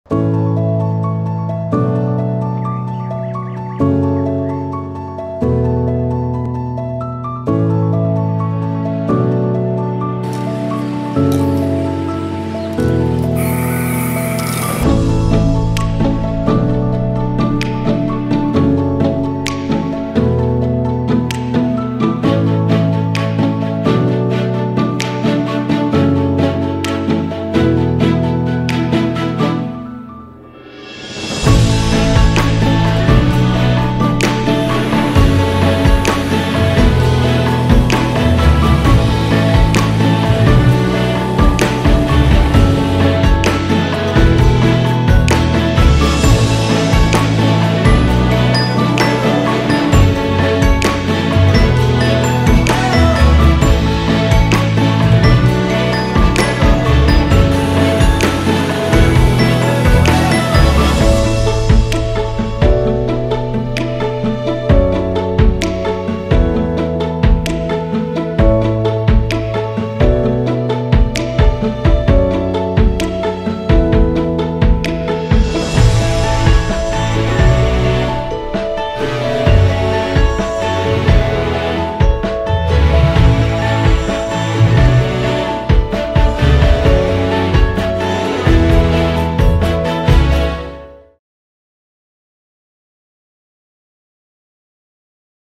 La régénération des terrains de sports - un nouveau souffle pour vos gazons sportifs
Zoom sur le scalpage des terrains hybrides (Mixto) des terrains annexes de la Licorne - Amiens (2022)